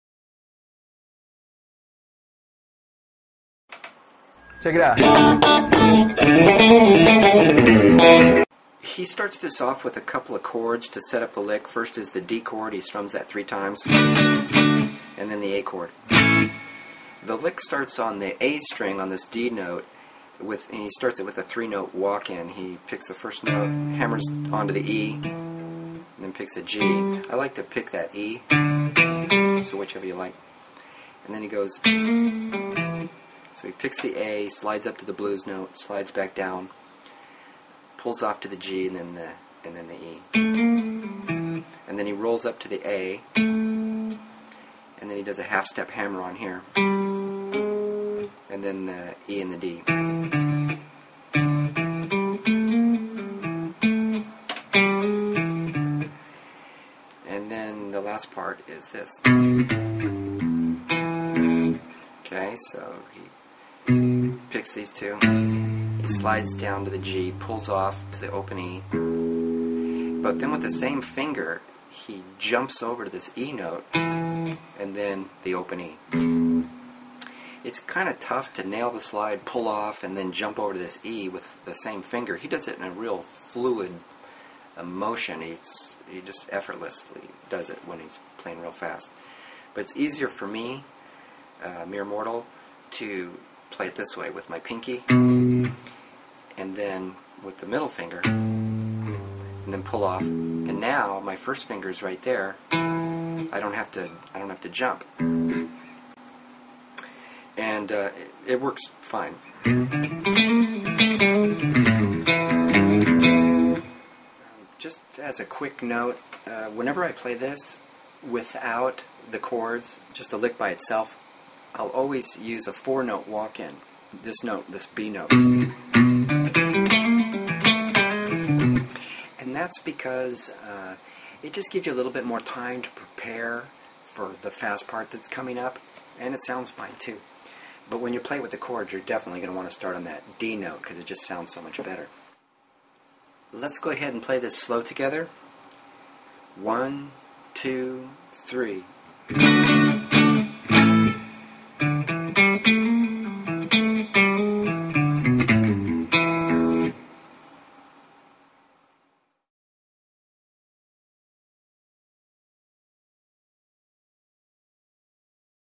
Phil X Lick! (Lesson)
Phill X Lick in E minor using the blues scale

Here's a link to the spot within the video where I got the lick:

I used YouTube's Video Editor to fix a problem I had with this video when I uploaded my video the audio cut out halfway through. So I re-uploaded the second half of the video...cut out the non-functioning portion of the original video, and reconnected the two parts. It's not a perfect/seamless transition, but it worked! You can see the glitch at 1:46 into the vid.